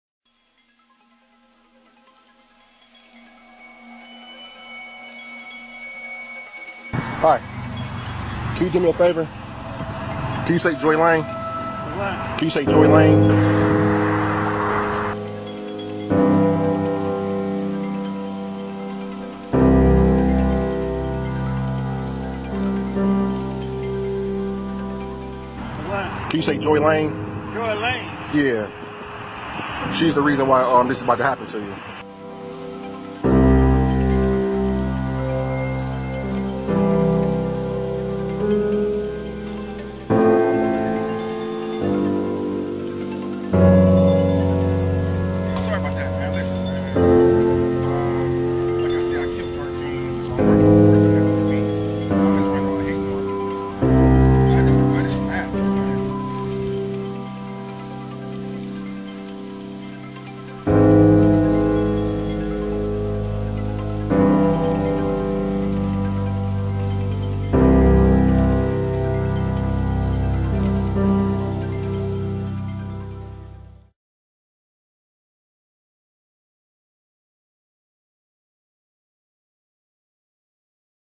After a police chase, the Facebook killer is found dead
Via PlayGround +